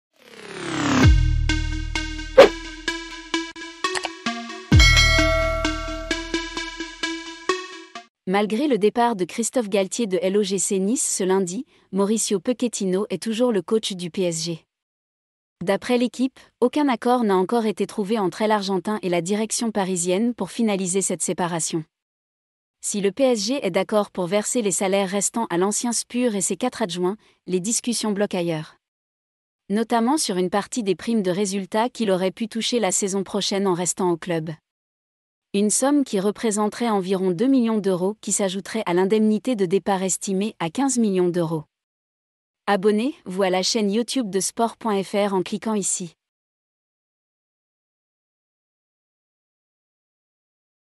Le PSG est complètement coincé avec Pochettino - actu foot
Malgré le départ de Christophe Galtier de l’OGC Nice ce lundi, Mauricio Pochettino est toujours le coach du PSG. D’après L’Equipe, aucun accord n’a encore été trouvé entre l’Argentin et la direction parisienne pour finaliser cette séparation. Si le PSG est d’accord pour verser les salaires restants à l’ancien Spur et ses quatre adjoints, les discussions bloquent ailleurs. Notamment sur une partie des primes de résultats qu’ils auraient pu toucher la...